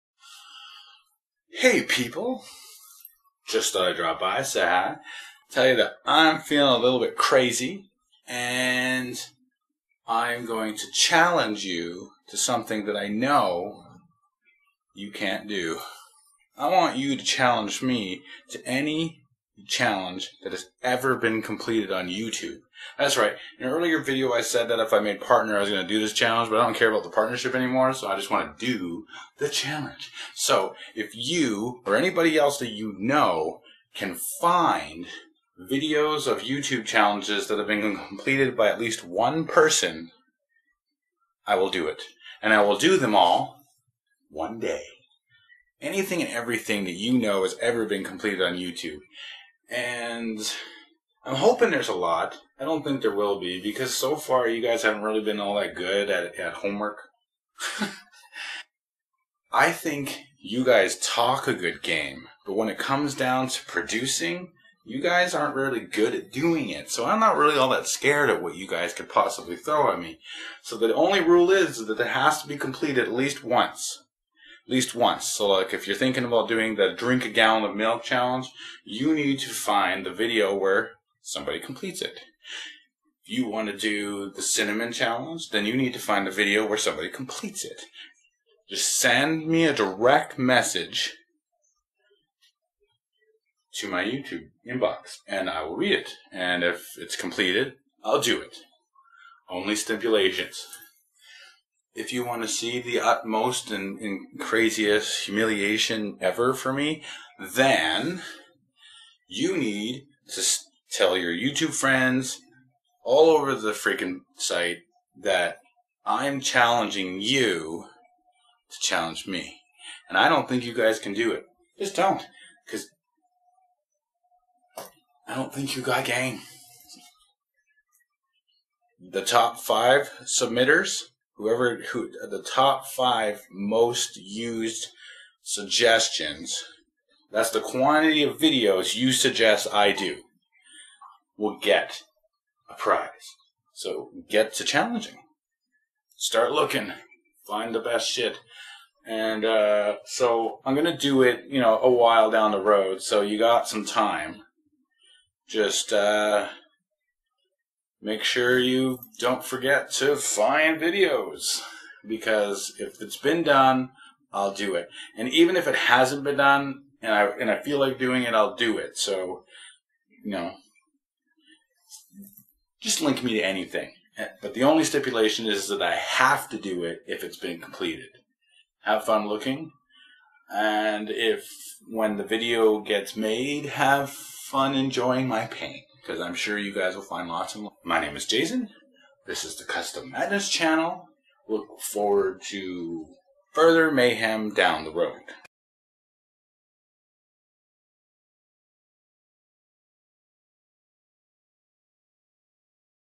Challenge Me U Can't Beat Me... I'M Mad Custom
NO MORE CHALLENGES CONTEST OVER!

Make a challenge to me by sending links to youtube video's that have been completed, and the only rule is that i HAVE to do the challenge if it has ever been completed. But seriously though i don't think you people have the follow through ambition, to make this hard for me, so when you guys leave comments like" "you'll never complete this" and "you can't beat that" i'll just say pffft...whatever you aint got game. WORD!

Challenges Accepted So Far

1. broadcast live on blogtv for 48 straight hours
2.cinnamon Challenge
3. drink 4 shots of lemon extract
4. watch 1 guy 1 jar without emotion
5. watch bme pain Olympics without emotion
6. watch 2 girl 1 cup
7. do the hoe down showdown
8. drink 1 cup of ketchup
9. drink a shot of vinegar
10.eat 1 piece of bread in under 60 secs
11. eat 5 crackers in under 60 secs
12. do an outdoor vlog
13. paint yourself purple
14. eat 42 pringles less 1 for crumbs
15. wax legs
16. table clothe challenge
17. 10 second fire challenge
18. put a lit cigarette out on your tongue
19. drink bottle of water in under 10 seconds
20. lick your own elbow
21. bounce 3 quarters off a bottle into cup
22. Try Not To Laugh! [Fun Challenge]
23. drink gallon of milk in 42 seconds
24. chubby bunny (again lol) 13